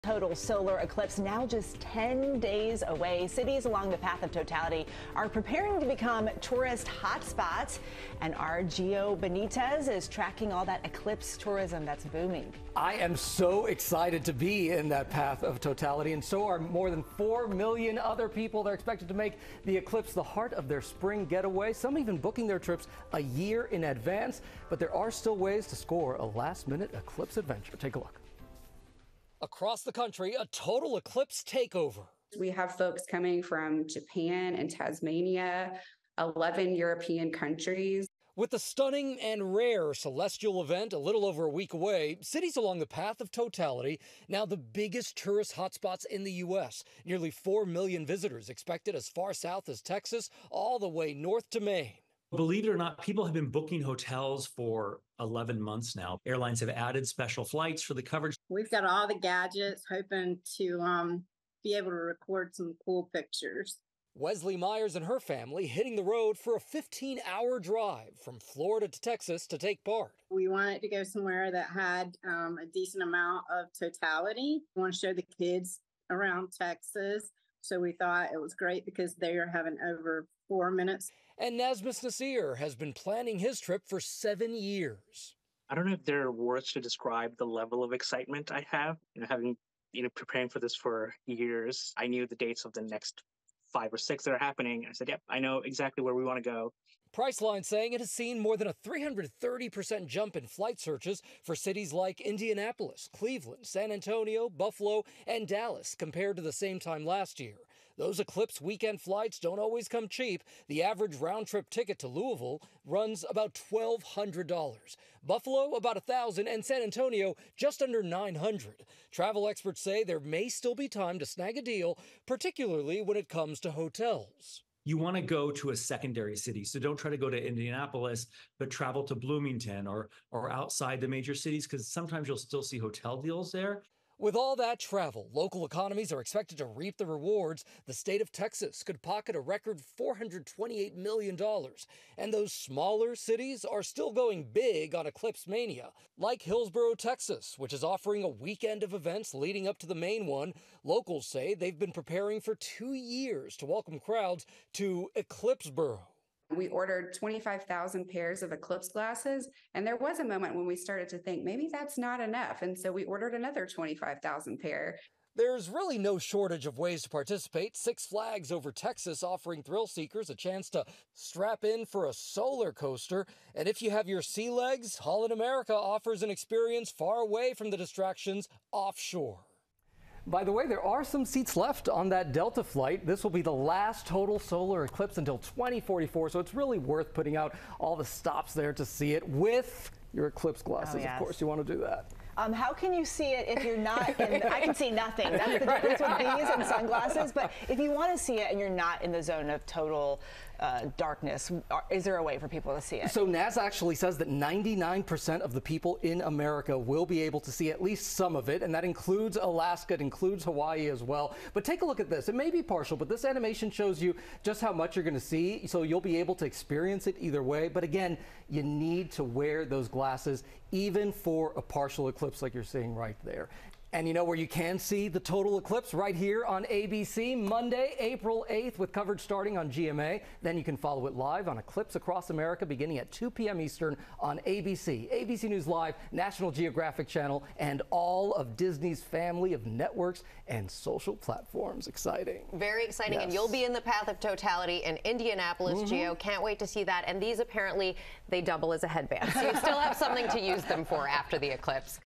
Cities along solar eclipse path prepare for influx of travelers
Areas along the path of totality are preparing to become the biggest hotspots in the U.S., with nearly 4 million visitors expected from Texas to Maine.